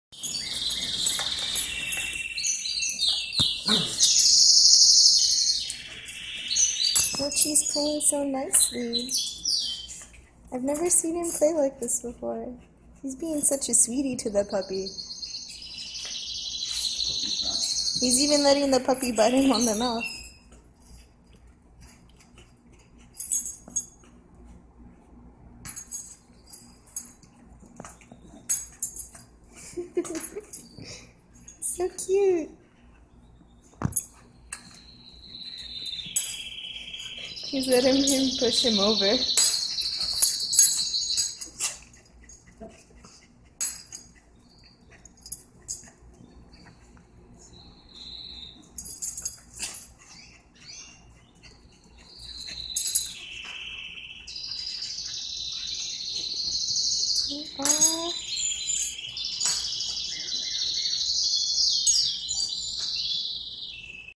FOSTER/RESCUE NEEDED!
This adorable puppy was found as a stray with his mom (who pops into the video for a brief cameo; mom is not the Corgi). Mom appears to be a 1.5-year old Chiweenie, and her son is about 3 months old. Mom walks well on a leash and may be housebroken; the pup has not had any training yet. Both enjoy being with people and other dogs. This friendly, happy pair needs a foster or rescue ASAP.